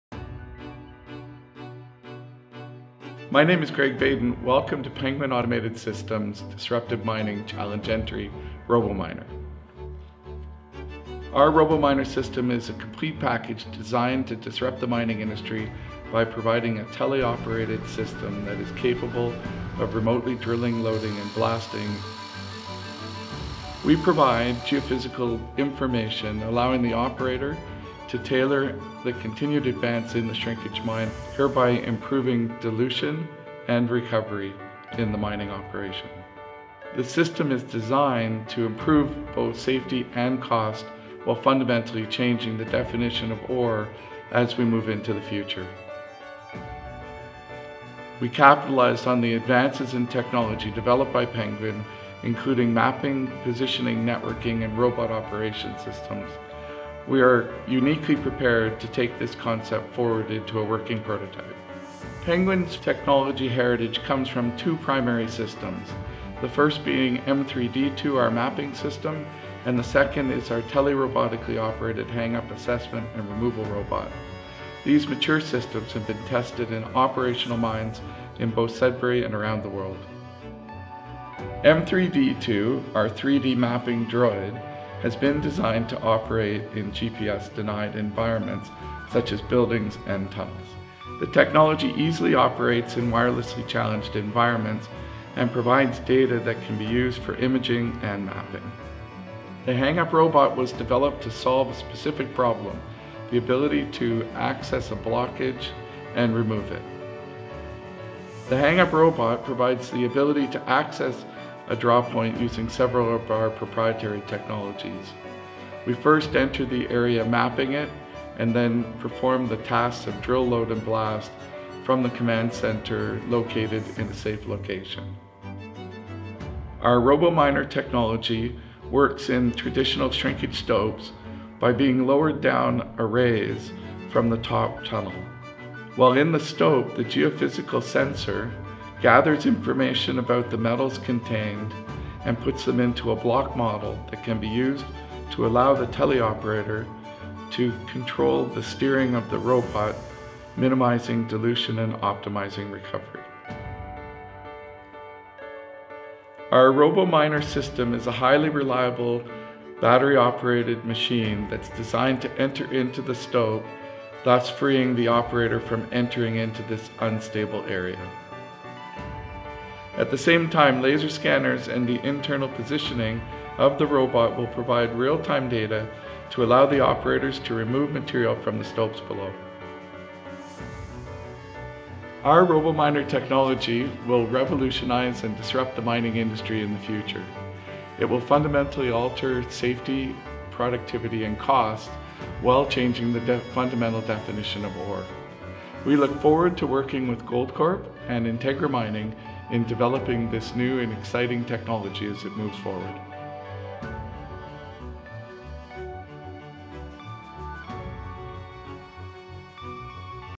Penguin-Braveheart Mining Robot Disruptor 2018
Penguin has conceived a new Robot System to dramatically improve safety and productivity using speciality networking, computing and advanced sensing systems combined with artificial intelligence techniques to determine mineral location